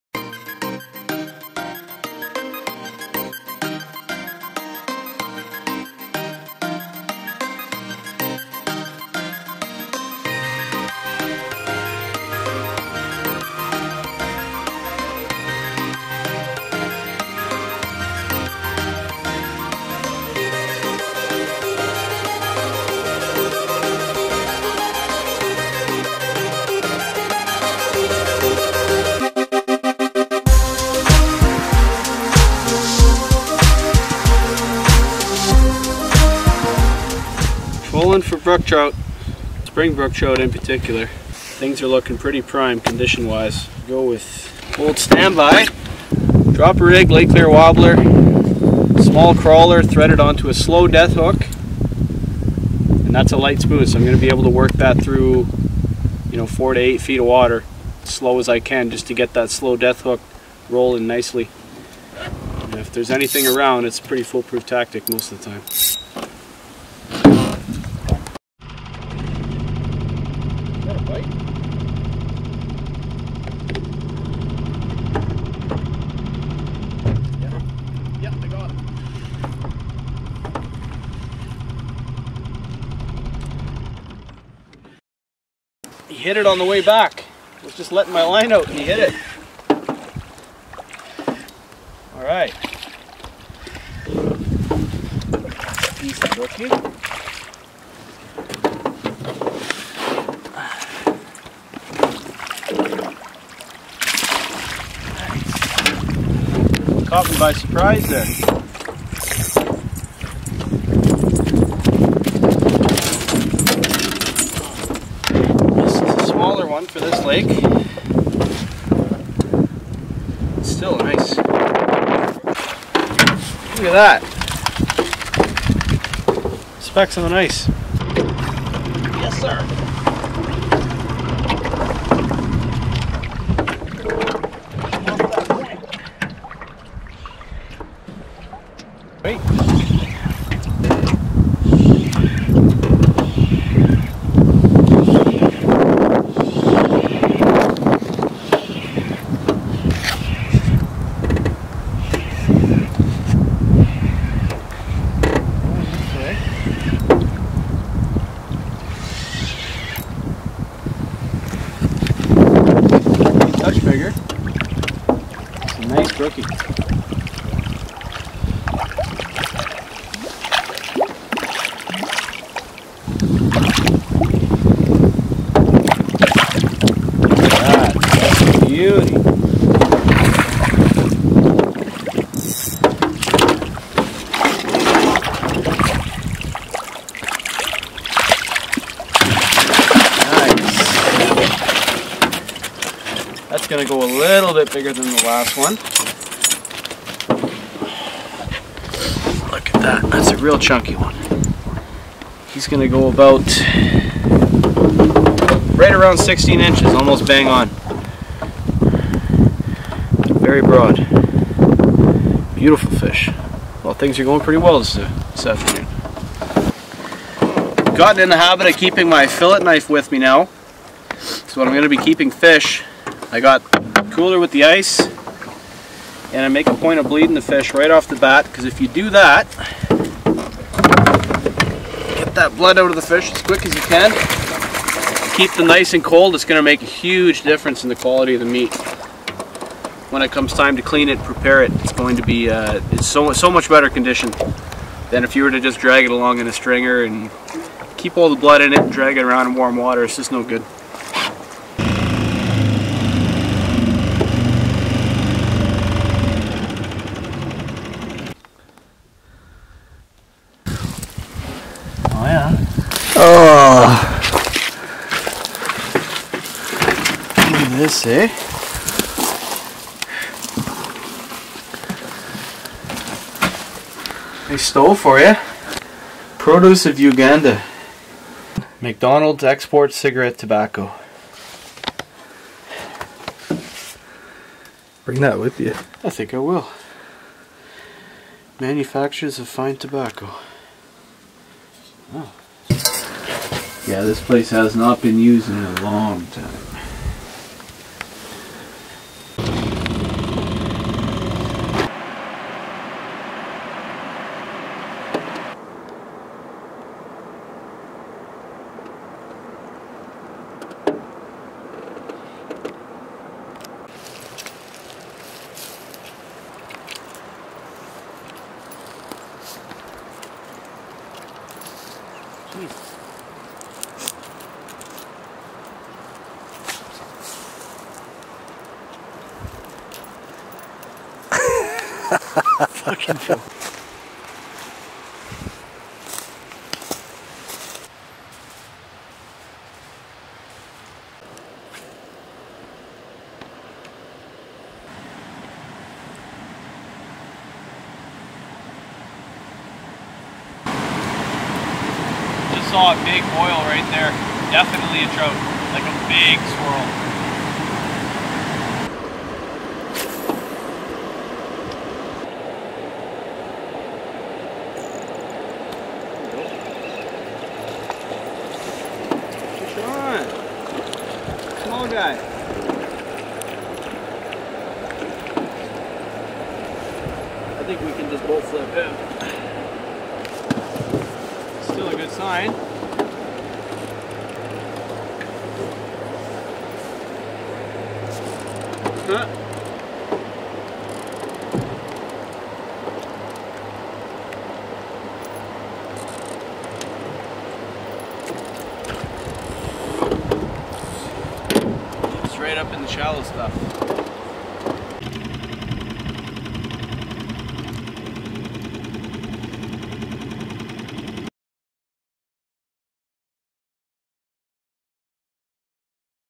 Exploring and Chasing Spring Brook Trout
Our favourite spring pastime is chasing brook trout, and the actual catching is only a small part of its appeal to most who venture out for these gems of the Canadian wilderness.